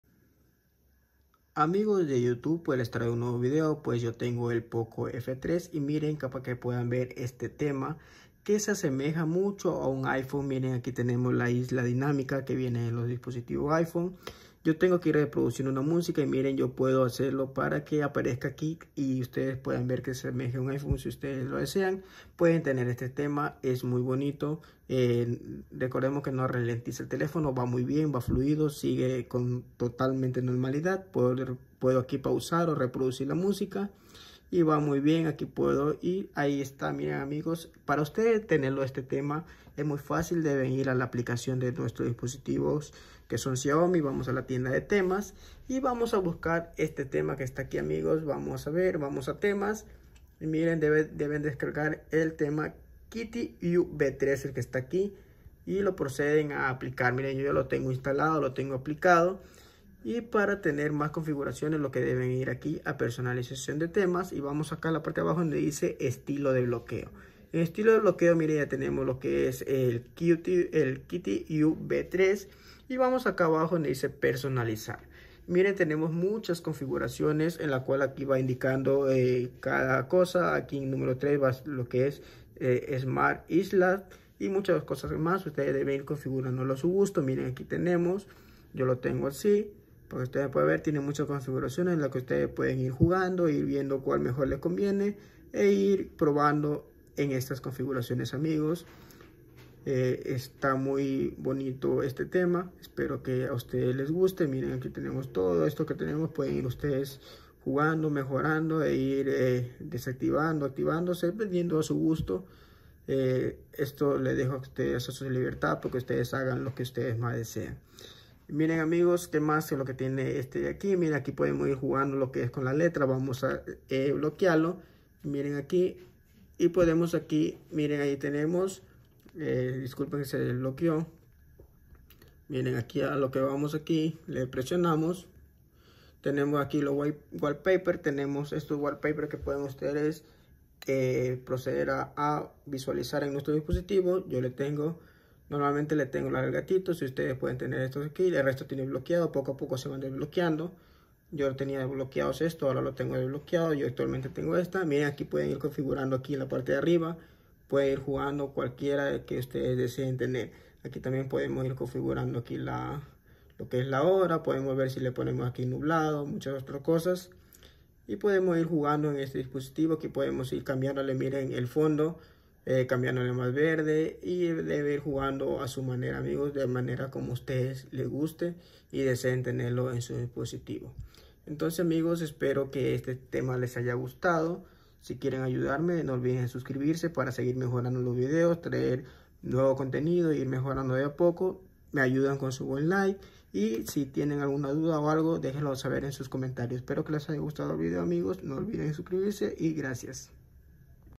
Nuevo tema completo para tu Xiaomi, instálenlo…
el tema es KittyUi V3 que está en la tienda de Temas, espero les guste el video